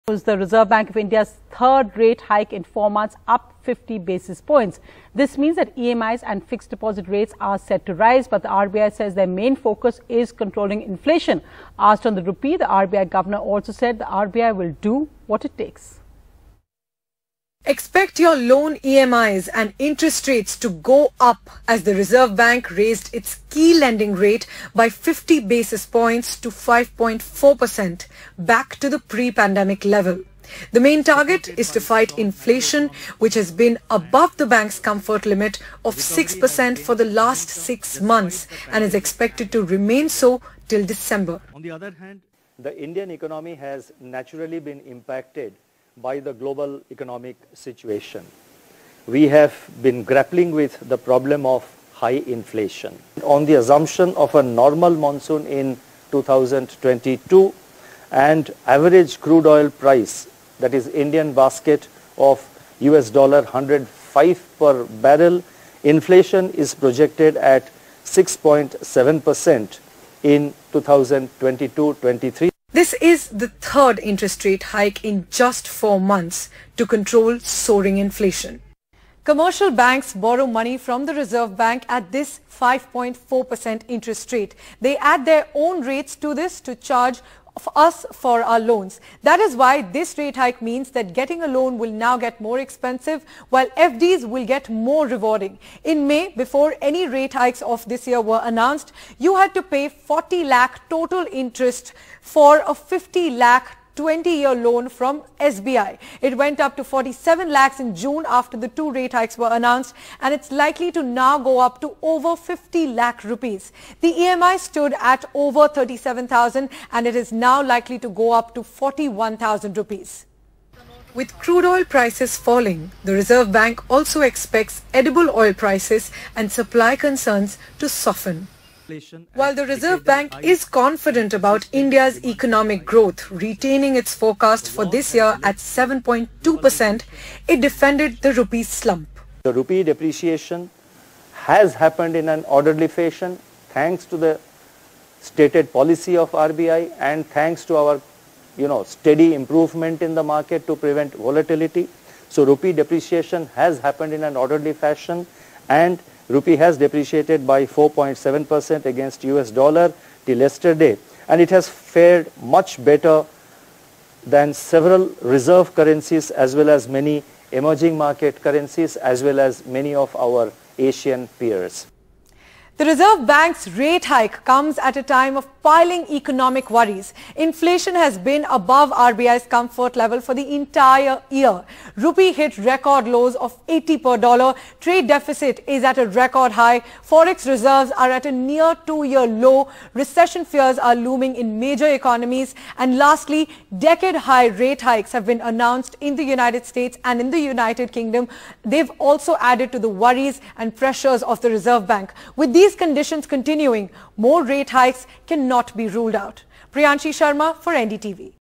EMIs To Rise As RBI Hikes Interest Rate Again | The News
Home, auto and other loan EMIs are set to rise further after RBI on Friday raised the key interest rate by 50 basis points, the third straight increase since May in an effort to cool stubbornly high inflation.